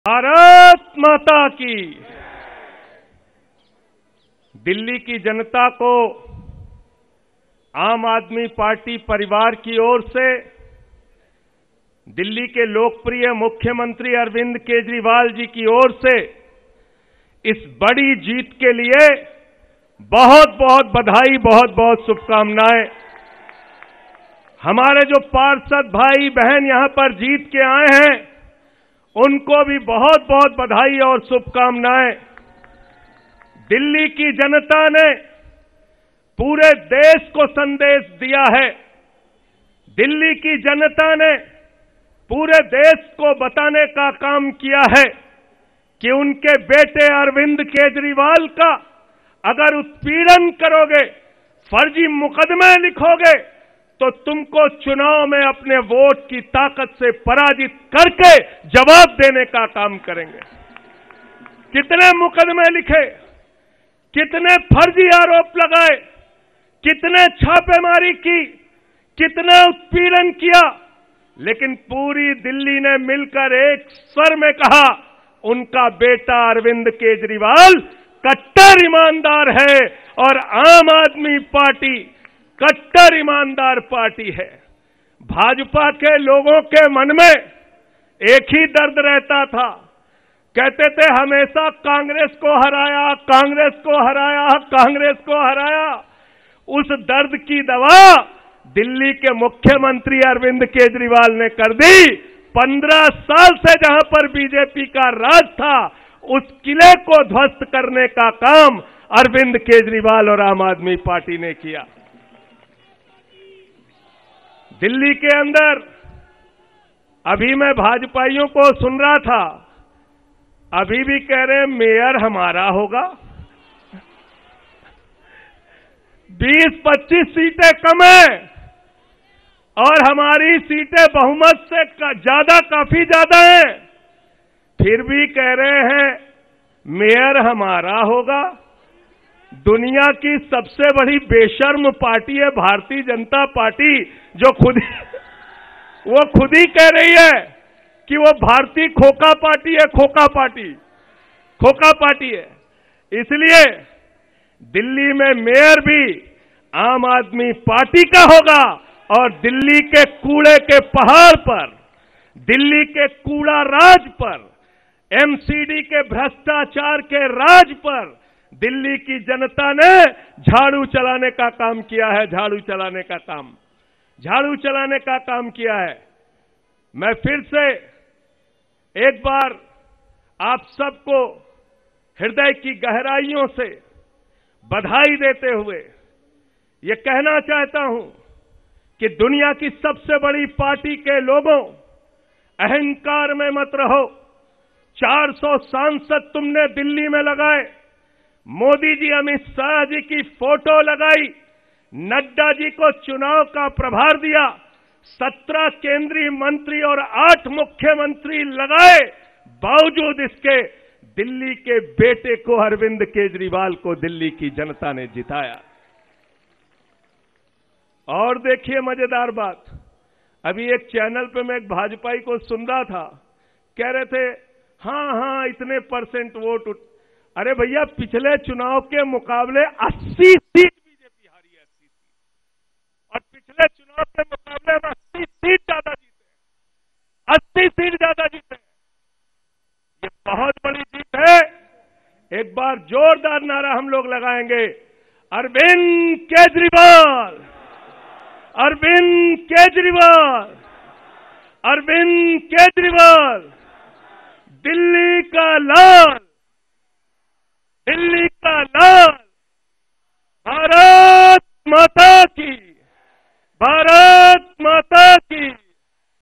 MCD Result 2022 : AAP नेता Sanjay Singh ने बताया, दिल्लीवालों ने BJP के किस दर्द का किया इलाज ?
MCD Election Result 2022 : AAP नेता Sanjay Singh ने बताया, दिल्लीवालों ने BJP के किस दर्द का किया इलाज ?

TV9 Delhi NCR Offical WhatsApp Chat Number | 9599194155